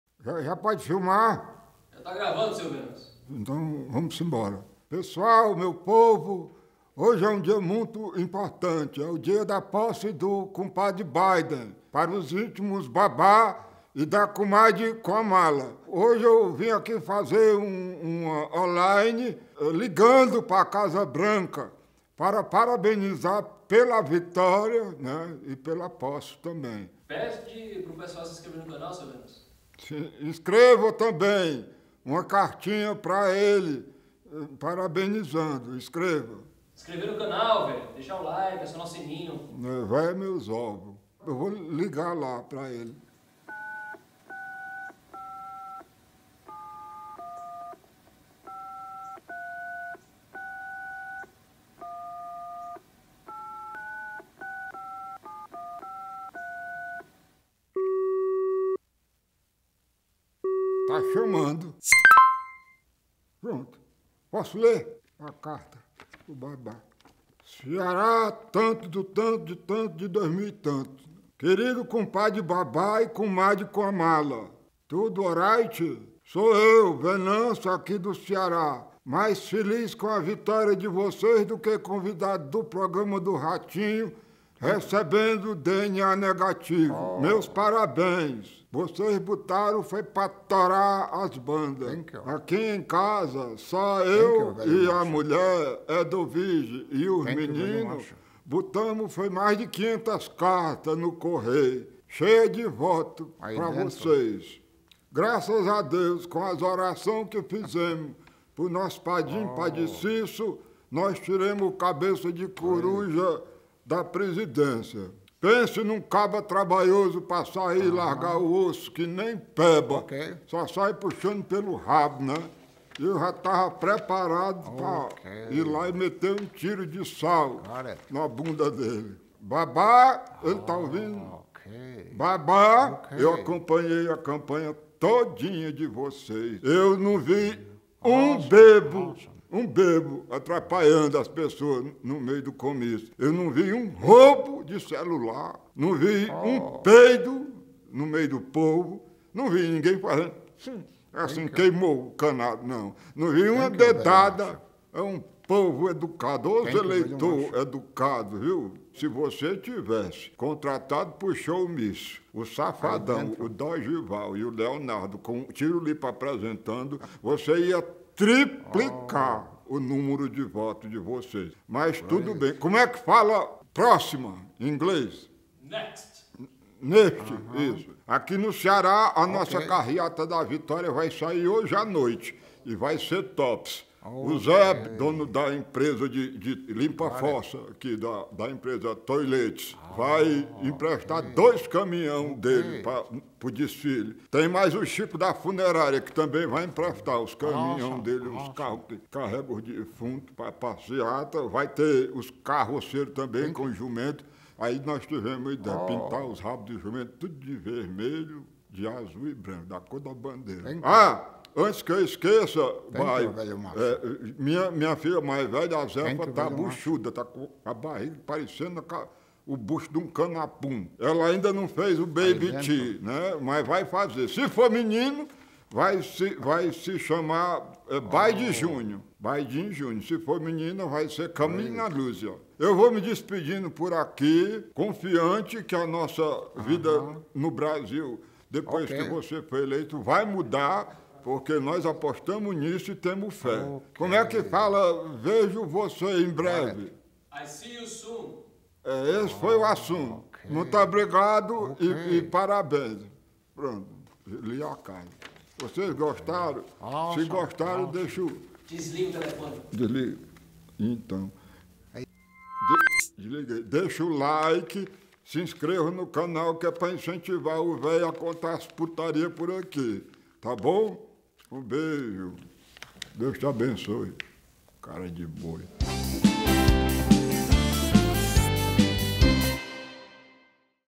Venancio cumprimenta o Cumpadi Biden
Vei Venâncio faz conferencia com o novo presidente dos Estados Unidos Biden, para cumprimentá-lo